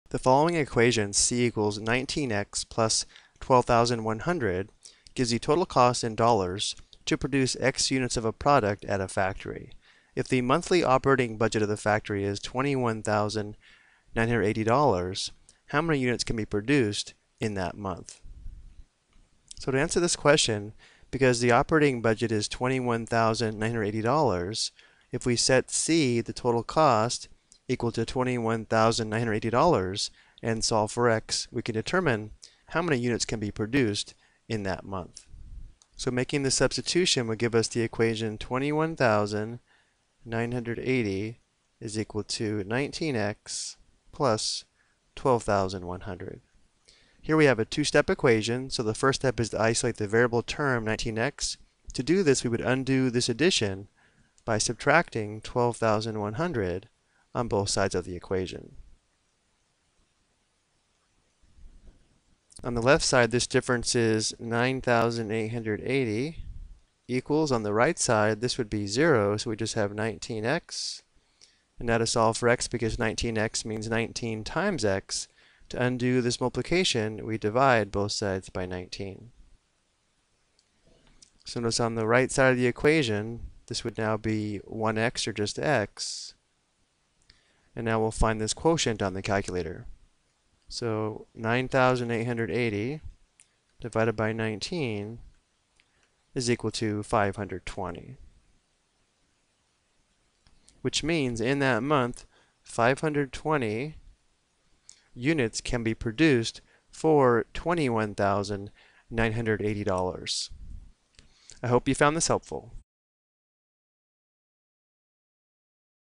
Ex: Given a Linear Cost Function and Bugdet, Find the Number of Items that Can be Produced
This video explains how to solve a linear equation to determine how many items can be produced given a budget and a cost  function.